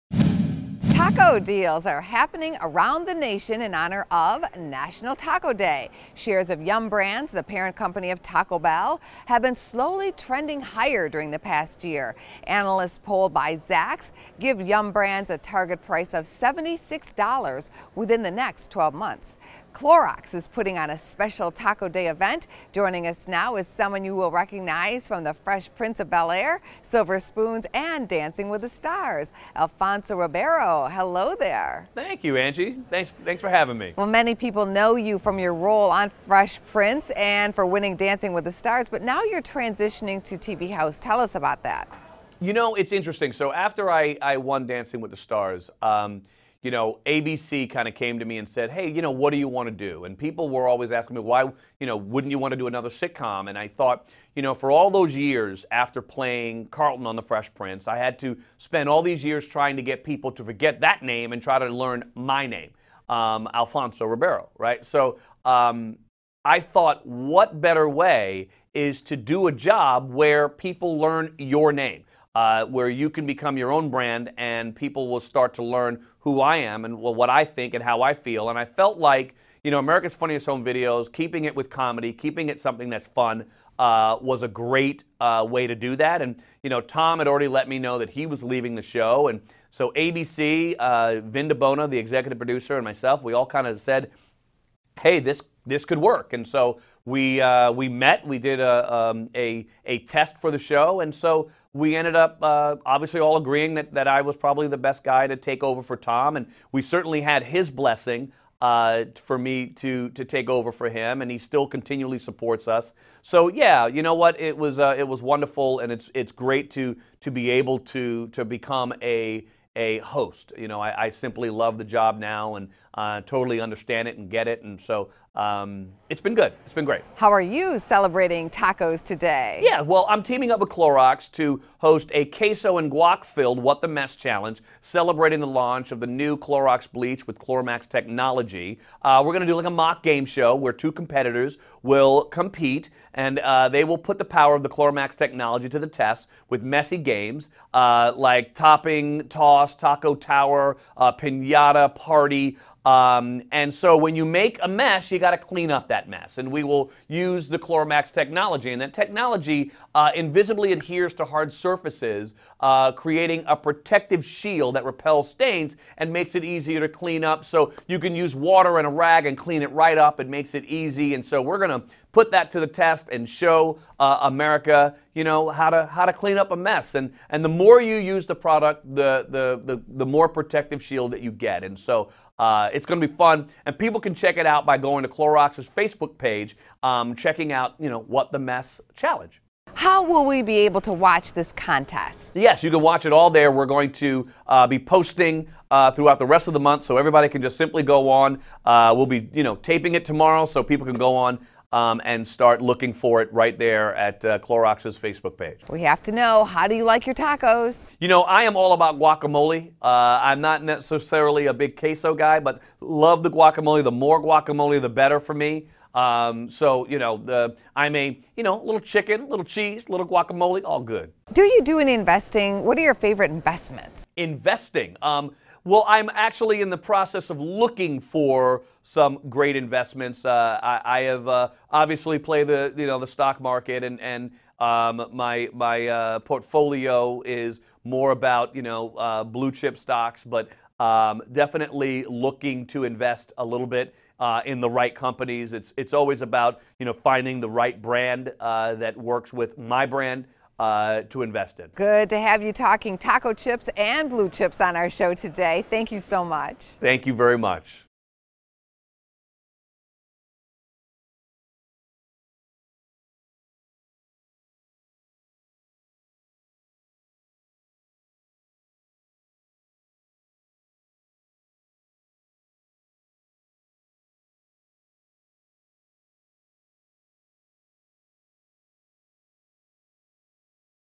TACO TRADE
Alfonso Ribeiro talks tacos for the national holiday, and a new contest from Clorox